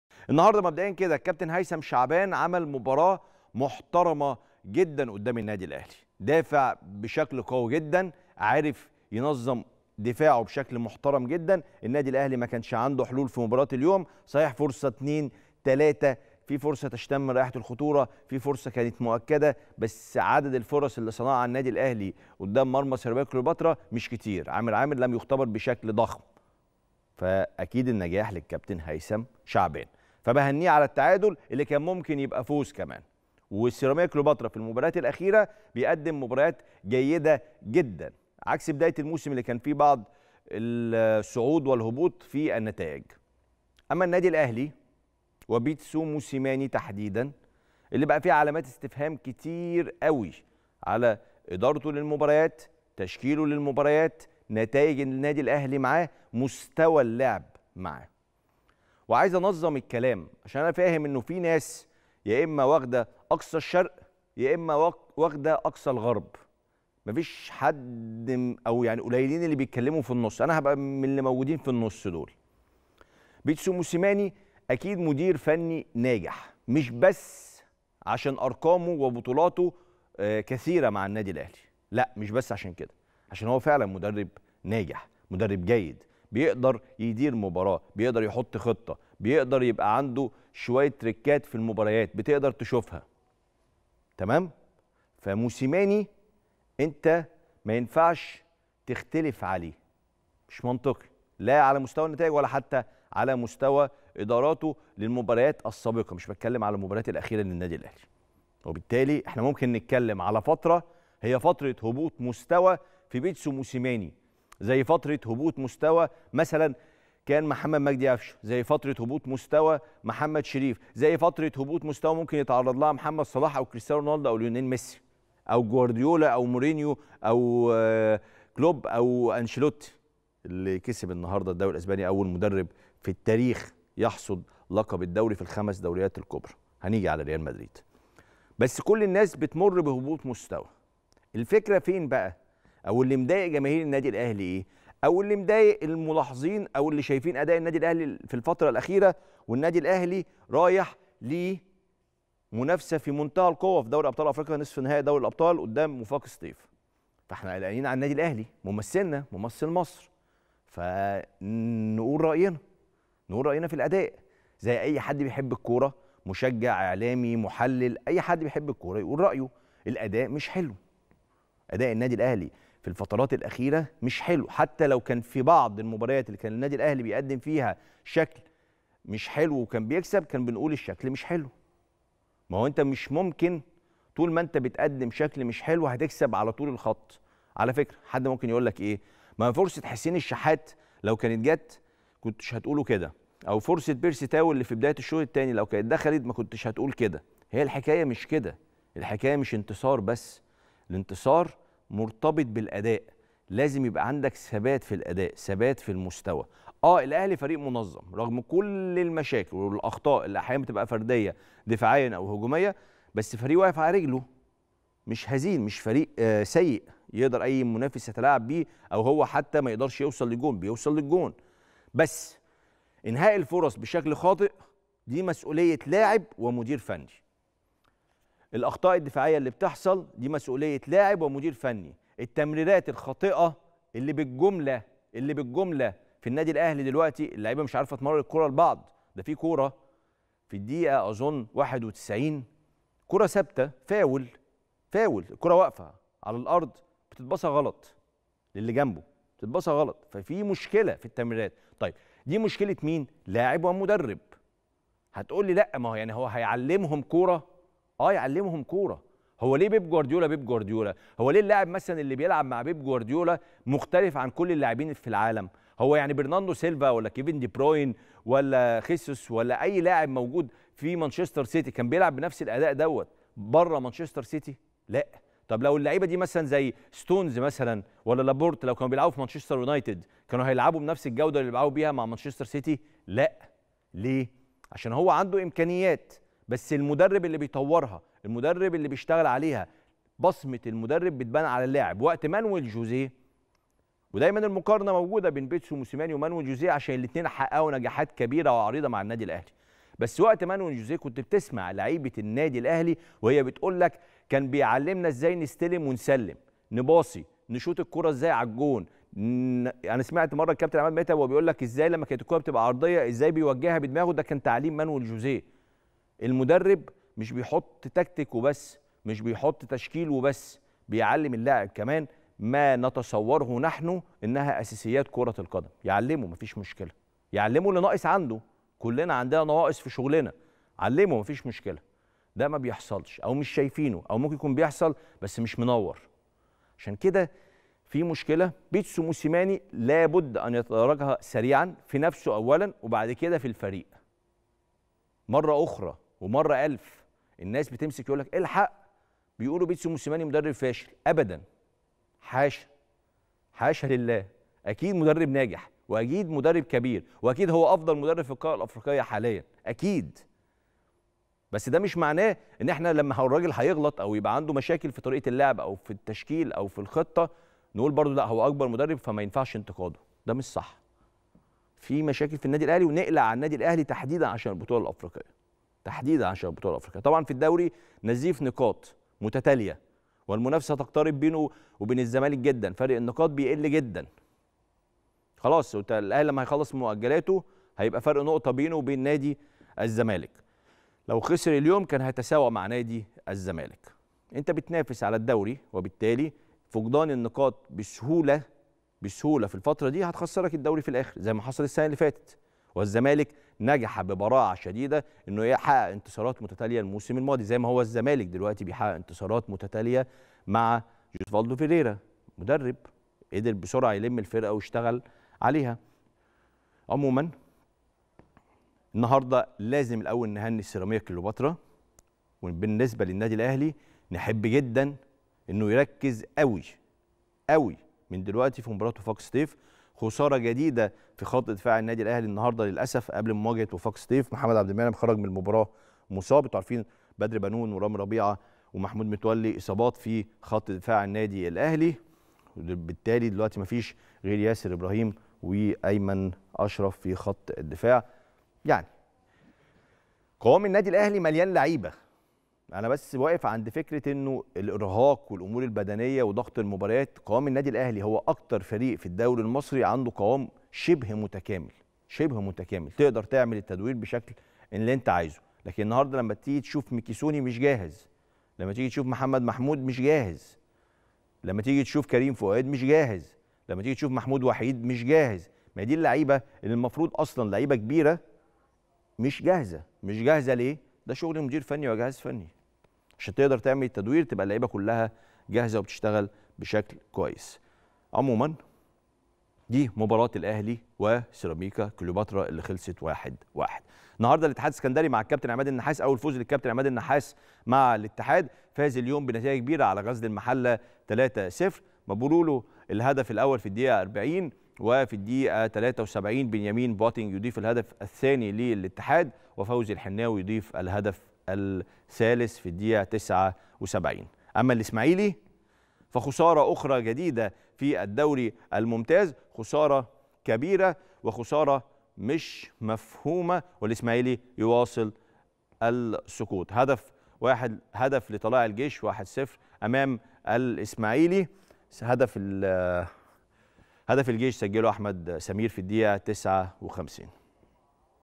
أداء الأهلي الفترة الأخيرة مش حلو .. مقدمة نارية من كريم رمزي بعد مباراة الأهلي وسيراميكا كليوباترا
"أداء النادي الأهلي في الفترة الأخيرة مش حلو" .. مقدمة نااارية من كريم رمزي بعد تعادل النادي الأهلي مع سيراميكا كليوباترا بالدوري المصري